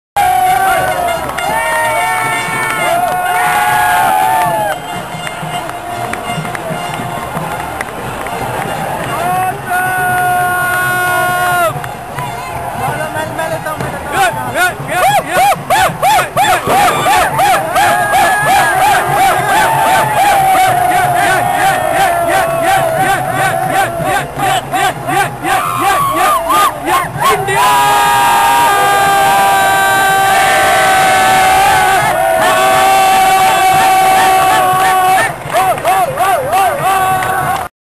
Team celebrations at Dubai.wmv
India wins WC.. Thanks to team India for giving this moment.